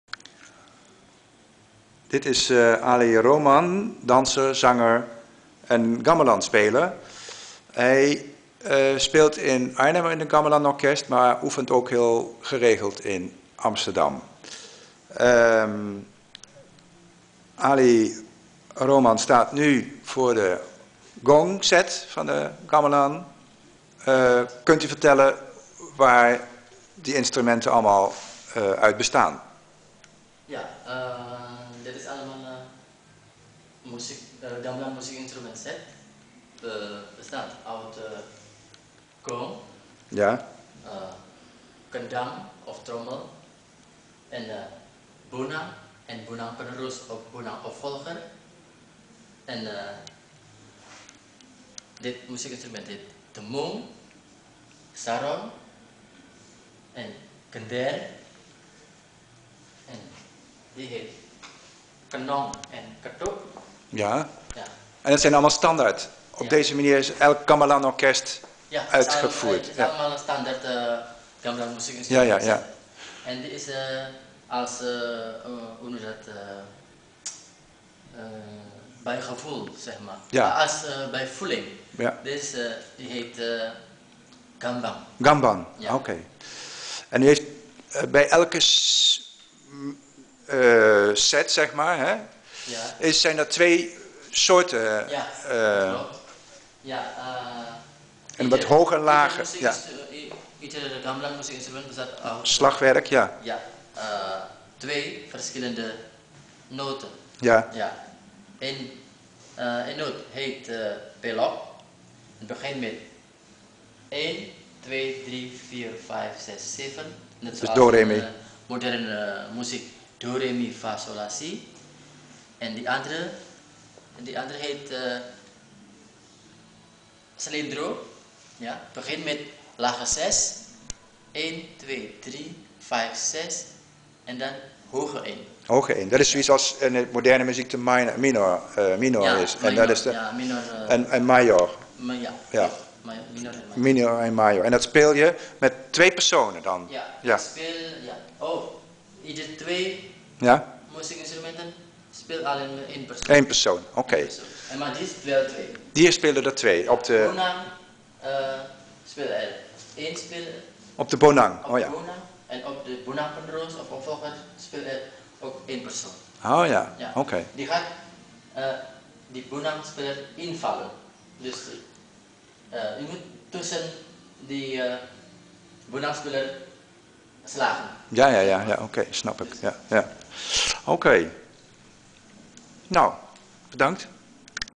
Gamelan Bronbeek
This is  Ali Rohman, who plays in the gamelan orchestra of Bronbeek in Arnhem.